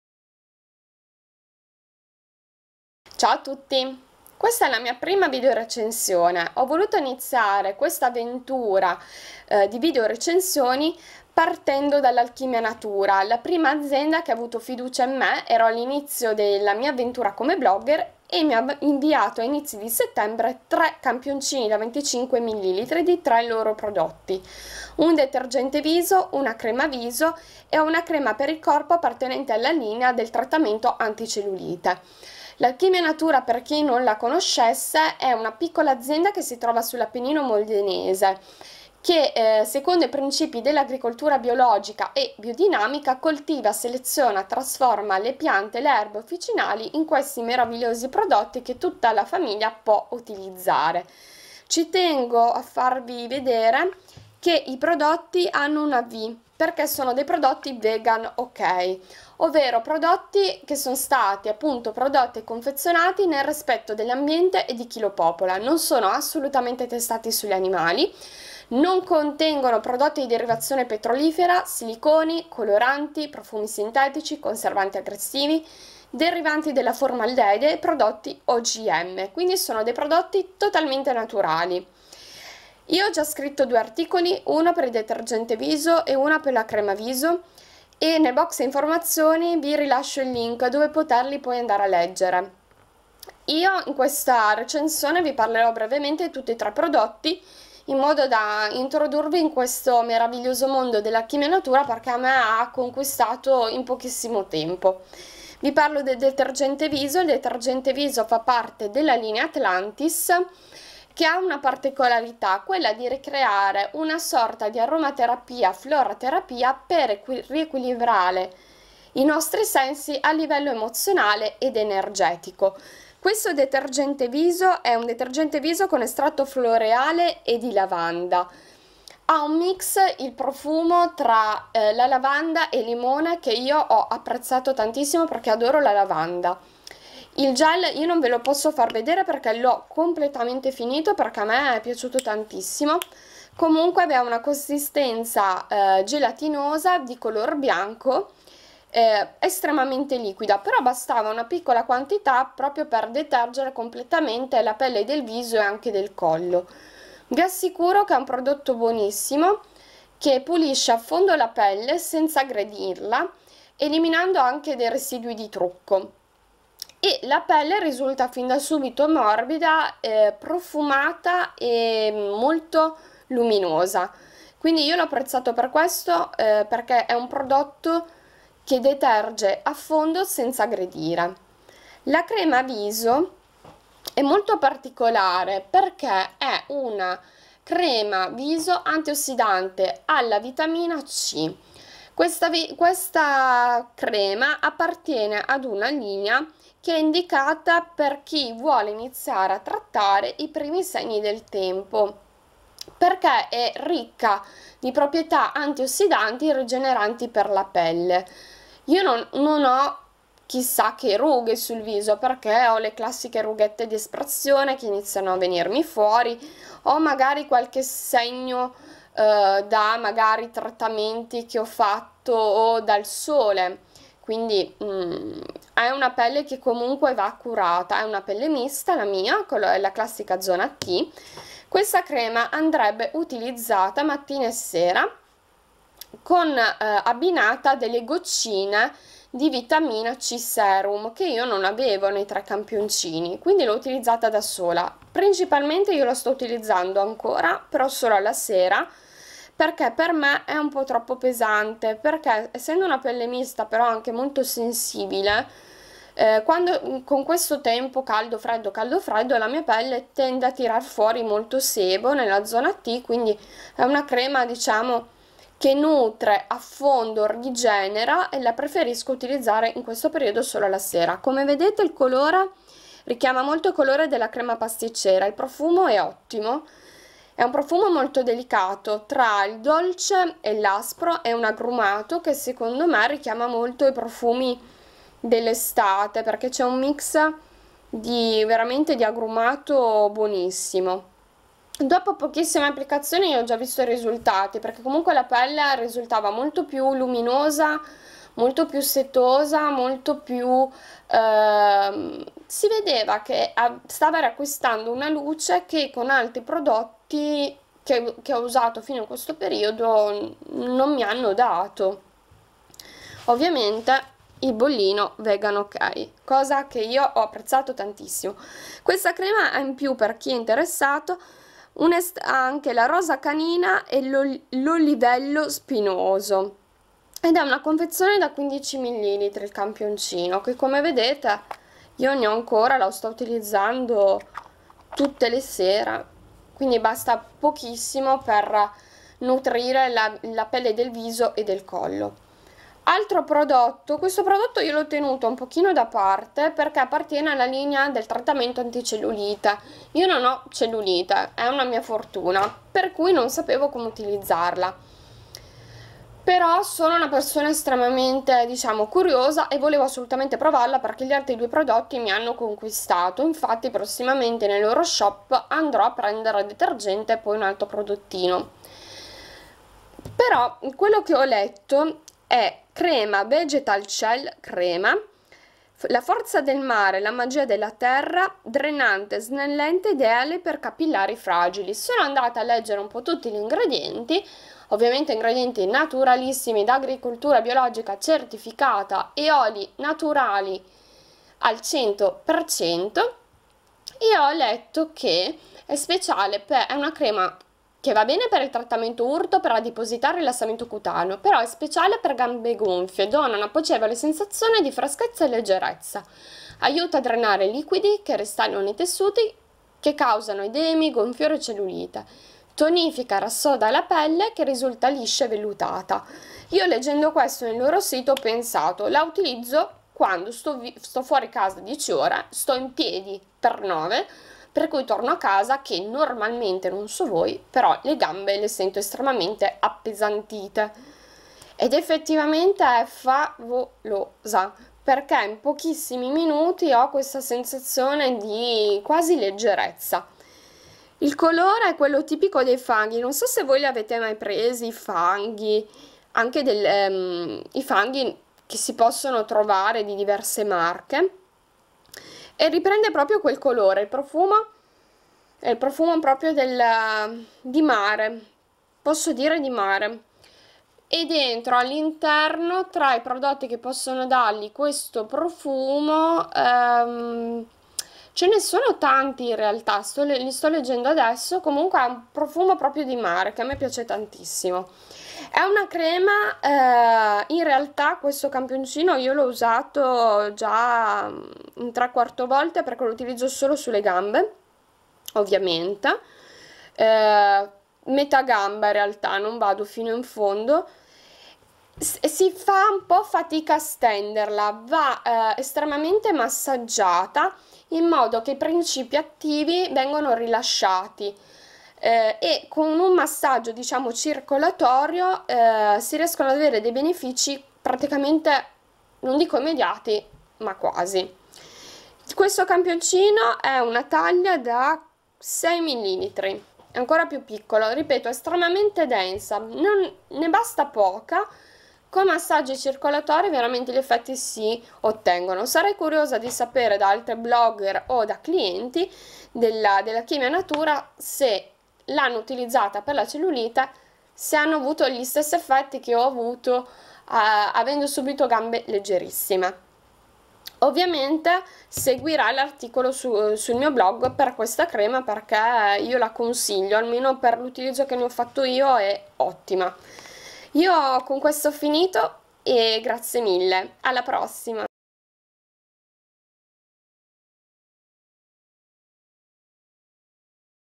RECENSIONE PRODOTTI ALCHIMIA NATURA s n c
Questa è la mia prima Video Recensione, spero di essere stata chiara e professionale ^_^
Vi lascio i link per leggere gli articoli correlati:

Per trasparenza, se ce ne fosse bisogno, nessuno mi ha pagato per parlare bene o male di un prodotto o dell'altro! Le opinioni espresse sono personalissime e derivate della loro applicazione, quindi dall'esperienza personale avuta, e soprattutto dal mio gusto.